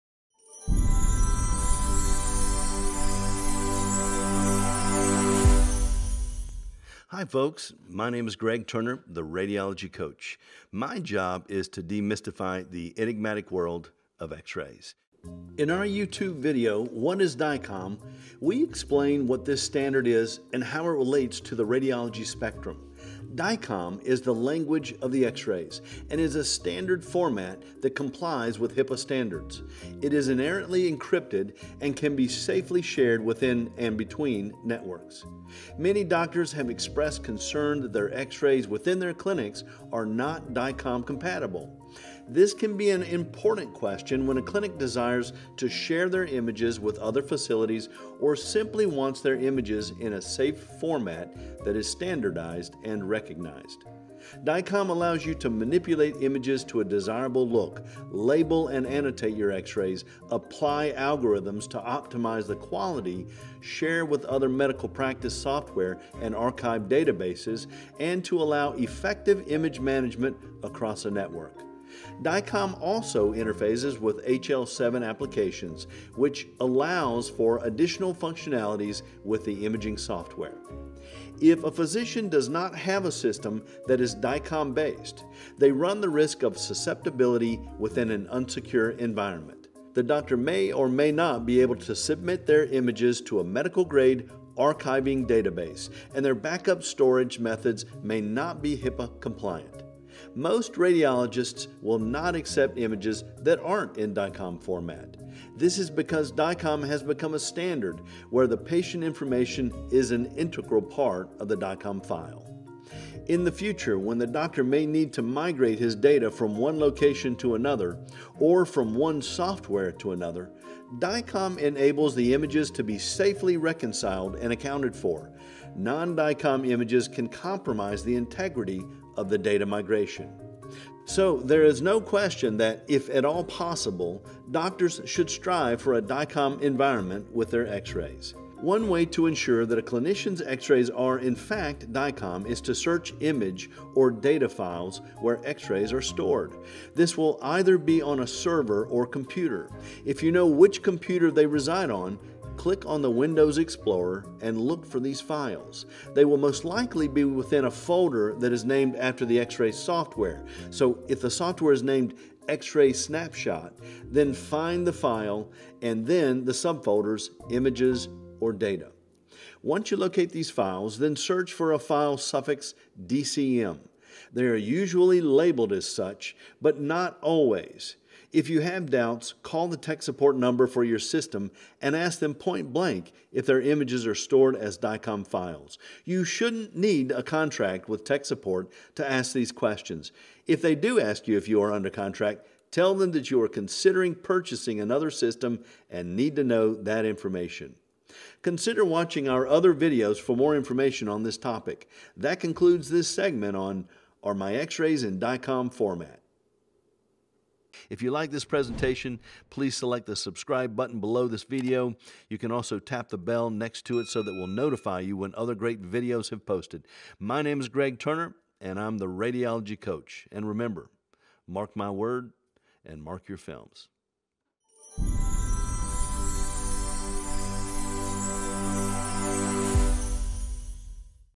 Are My X-RAYS in DICOM Format??
Physicians are occasionally confronted with the prospect of whether or not their x-rays are in DICOM format. This is a loaded question, as many doctors and managers may not understand the full implications of DICOM vs. non-DICOM. The Radiology Coach illuminates the reasons for considering this as a standard.